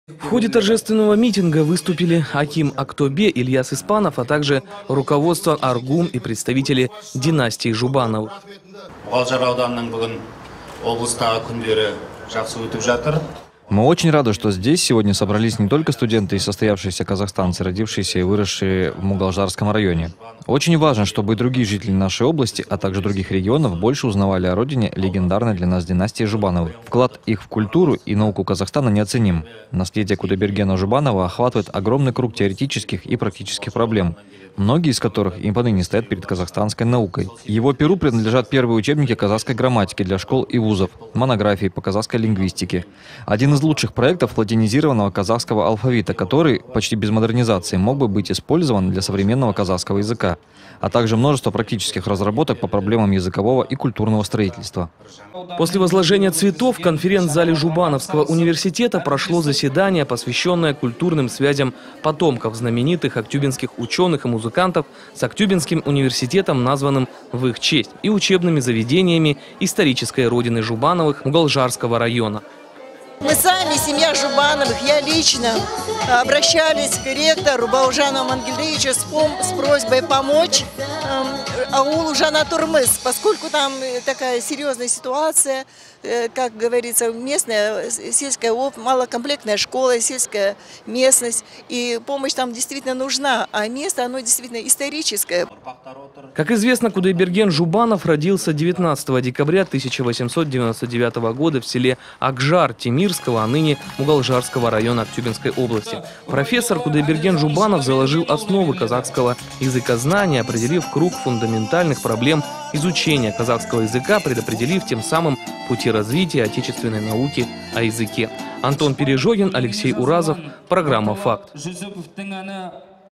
В Актобе сегодня прошло чествование знаменитой династии Жубановых
В Актобе сегодня прошло чествование знаменитой династии Жубановых. Оно состоялось в рамках проведения Дней Мугалжарского района в областном центре. В Актюбинском региональном государственном университете имени Кудайбергена Жубанова прошло торжественное возложение цветов к памятнику знаменитого земляка студентами из Мугалжарского района. В ходе торжественного митинга выступили аким Актобе Ильяс Испанов, а также руководство АРГУ и представители династии Жубановых. После возложения цветов в конференц-зале университета прошло заседание, посвященное культурным связям потомков знаменитых актюбинских ученых и музыкантов с актюбинским университетом, названным в их честь и учебными заведениями исторической родины Жубановых - Мугалржарского района.
Как известно, Кудайберген Жубанов родился 19 декабря 1899 года в селе Акжар, Темирского (ныне Мугалжарского) района Актюбинской области. Профессор Кудайберген Жубанов заложил основы казахского языкознания, определив круг фундаментальных проблем изучения казахского языка, предопределив тем самым пути развития отечественной науки о языке.